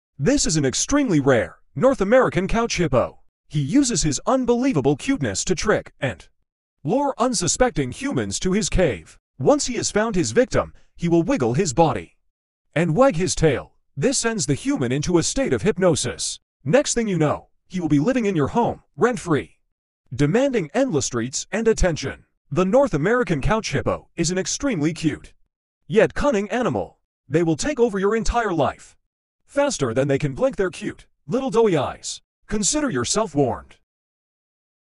PSA! North American Couch Hippo! 🌎🛋🦛
Warning, Beware! This is a PSA on the North American Couch Hippo! They are extremely tricky and cunning. Before you know it they will take over your home and life, forever!